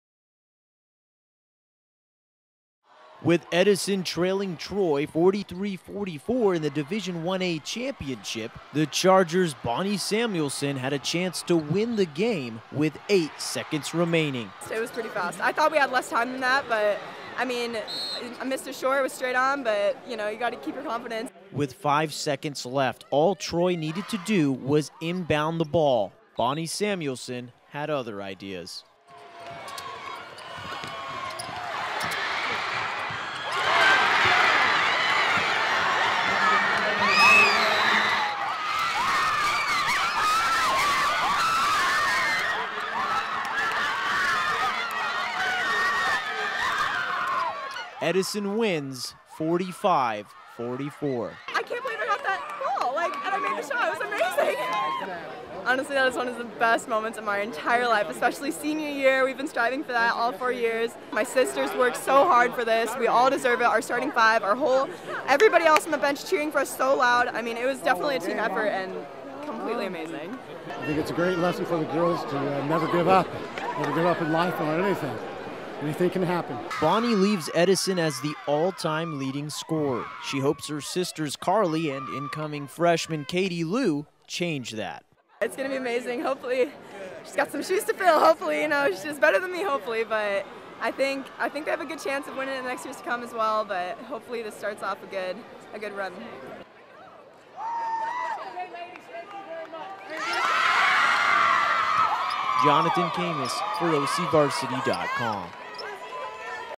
Edison's Samuelson shocks Troy in improbable finish
Edison's Bonnie Samuelson shocked Troy by stealing the inbounds pass and sinking the game-winning shot. Edison went on to defeat Troy 45-44 in the Division 1A championship game. Video by Jonathan Khamis, OCVarsity.com